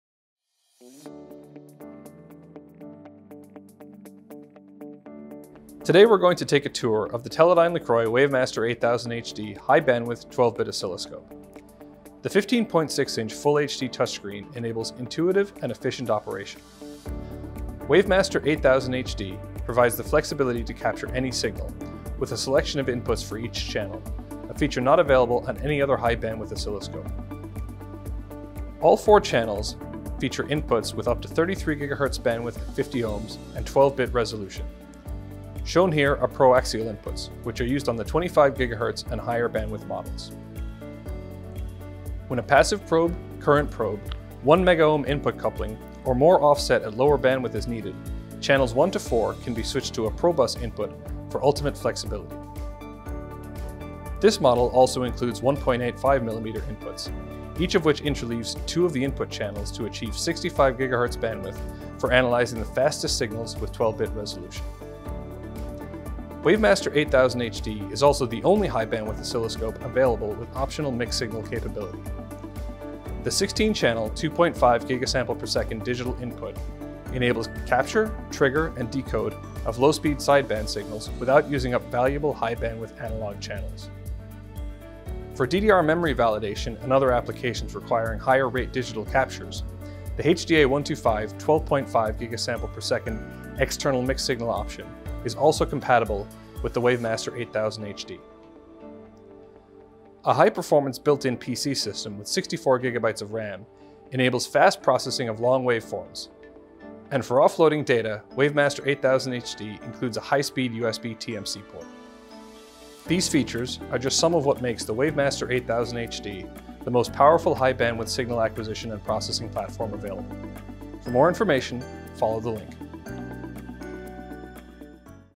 WaveMaster 8000HD Oscilloscope: Product Overview
WaveMaster 8000HD high speed oscilloscopes have high bandwidth (up to 65 GHz) and 12 bits of resolution all the time with 1 Megohm inputs and MSO options.

In this video we're going to take a tour of the Teledyne LeCroy WaveMaster 8000HD high-bandwidth 12-bit oscilloscope. The WaveMaster 8000HD features a 15.6" full-HD touchscreen, enabling intuitive and efficient operation. WaveMaster 8000HD provides the flexiblity to capture any signal, with a selection of inputs for each channel - a feature not available on any other high-bandwidth oscilloscope. All four input channels feature high-bandwidth inputs for up to 33 GHz bandwidth at 50Ω with 12 bit resolution.

This model also includes 1.85mm inputs, each of which interleaves two of the input channels to achieve 65 GHz bandwidth for analyzing the fastest signals with 12 bit resolution. WaveMaster 8000HD is also the only high-bandwidth oscilloscope available with optional mixed-signal capability. The 16-channel, 2.5 GS/s digital input enables capure, trigger and decode of low-speed sideband signals without using up valuable high-bandwidth analog channels.

A high-performance built-in PC system with 64 GB of RAM enables fast processing of long waveforms, and for offloading data, WaveMaster 8000HD includes a high-speed USBTMC port. These features are just some of what makes the WaveMaster 8000HD the most powerful high bandwidth signal acquisition and processing platform available.